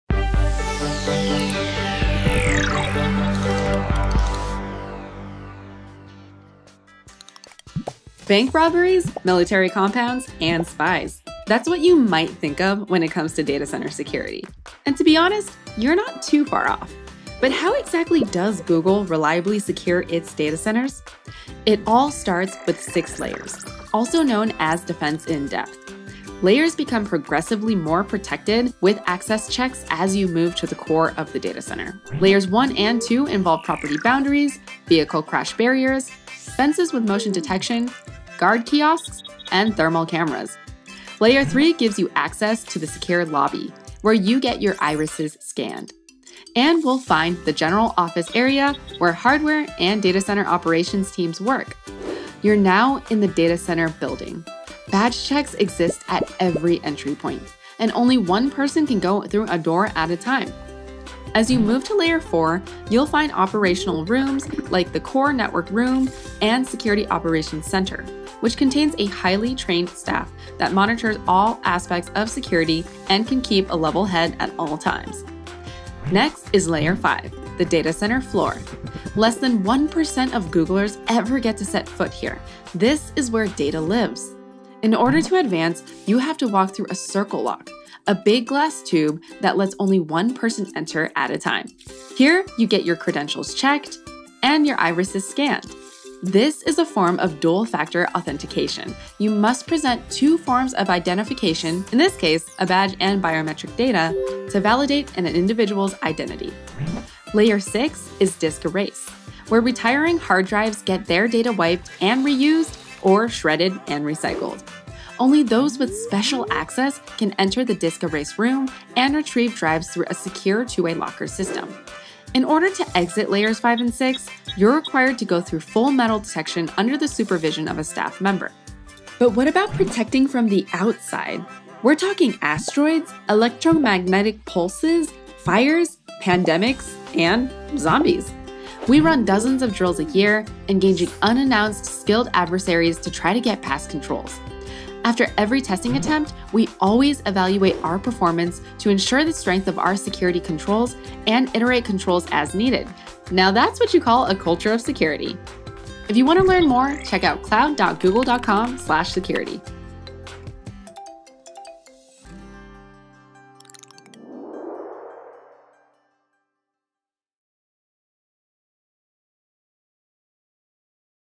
How does Google secure its data centers?
What does securing a Google data center really look like? In this episode of Discovering Data Centers, Stephanie Wong will guide you through the six layers of security that protects Google’s data centers from threats. From fences to biometric scanners and testing attempts, we’ll show you how security is a culture when it comes to protecting sensitive data.

Timestamps:
0:00 - Intro
0:30 - Layers 1 & 2
0:40 - Layer 3
1:01 - Layer 4
1:15 - Layer 5
1:46 - Layer 6
2:08 - Testing attempts

Product: Google Data Centers; fullname: Stephanie Wong;